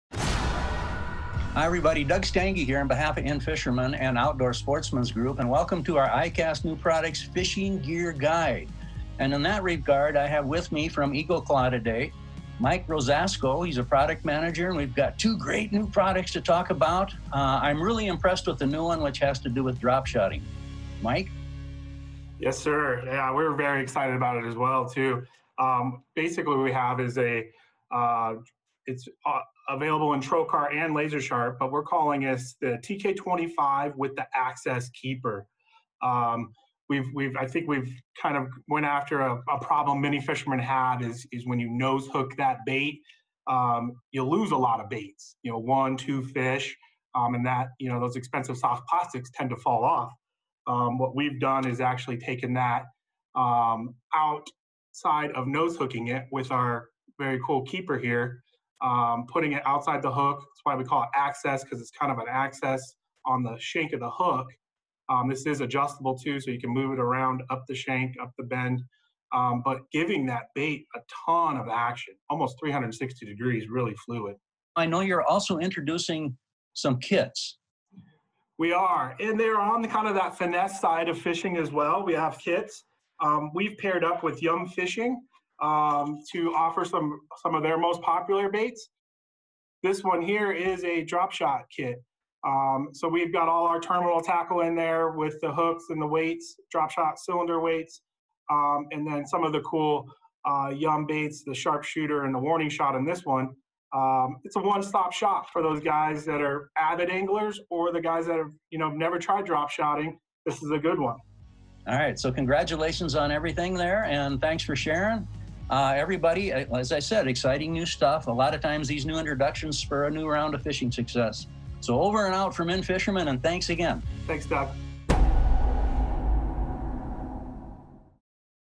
Eagle Claw AXS Dropshot Hook & Lazer Sharp Pro Series Avid Kits
Finesse anglers will love these new products from Eagle Claw; ICAST Fishing Gear Guide.